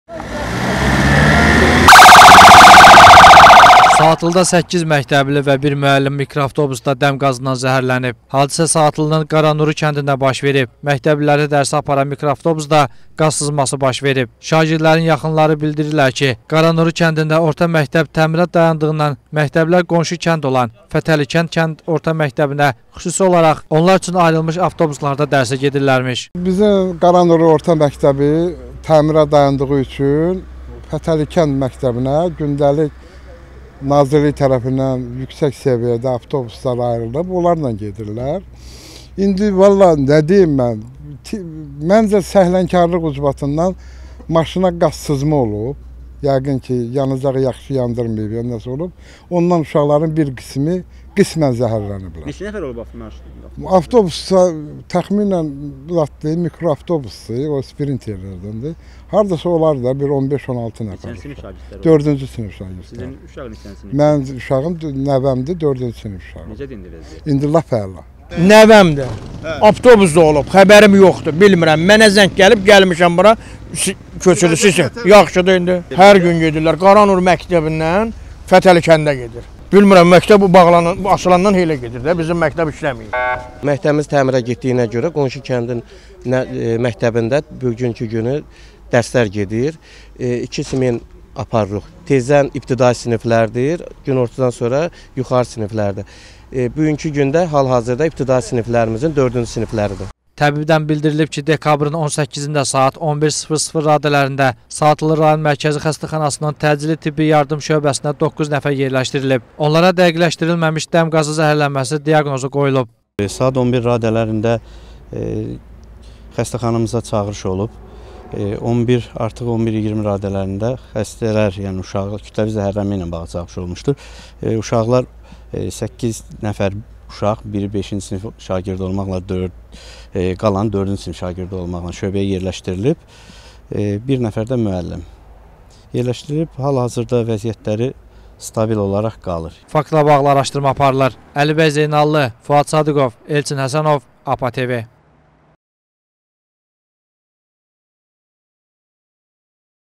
Saatlıda 8 məktəbli və 1 müəllim avtobusda qazdan zəhərləndi - APA TV
Saatlıda 8 məktəbli və 1 müəllim mikroavtobusda dəm qazından zəhərlənib.

Hadisə Saatlının Qaranuru kəndində baş verib. Məktəbliləri dərsə aparan mikroavtobusda qaz sızması baş verib.

Şagirdlərin yaxınları bildirir ki, Qaranuru kəndində orta məktəb təmirə dayandığından məktəblilər qonşu kənd olan Fətəlikənd kənd orta məktəbinə xüsusi olaraq onlar üçün ayrılmış avtobuslarla dərsə gedirlərmiş.

TƏBİB-dən bildirilib ki, Dekabrın 18-də saat 11:00 radələrində Saatlı Rayon Mərkəzi Xəstəxanasının Təcili tibbi yardım şöbəsinə 9 nəfər yerləşdirilib. Onlara dəqiqləşdirilməmiş dəm qazı zəhərlənməsi diaqnozu qoyulub.

Əlibəy Zeynallı
Elçin Həsənov